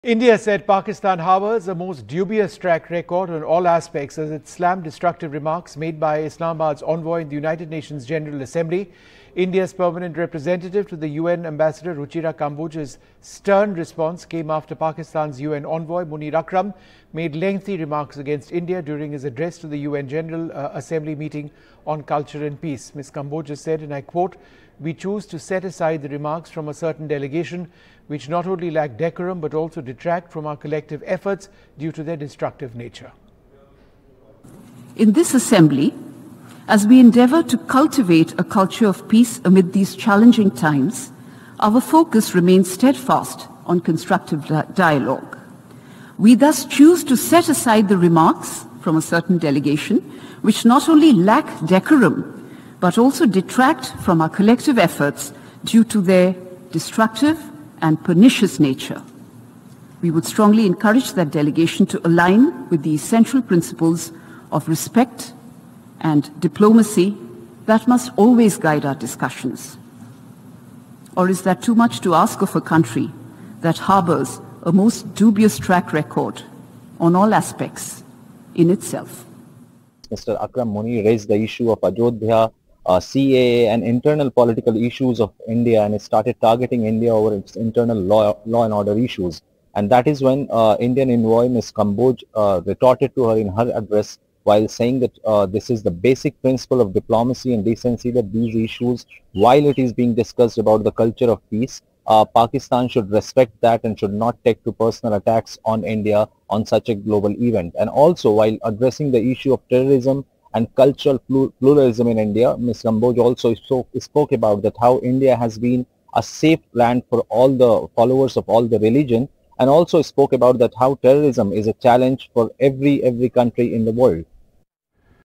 India At UN General Assembly: Pak Harbours "Most Dubious Track Record" On All Aspects
In a sharp retort, India has said Pakistan harbours a "most dubious track record" on all aspects, as it slammed "destructive and pernicious" remarks made by Islamabad's envoy in the UN General Assembly here.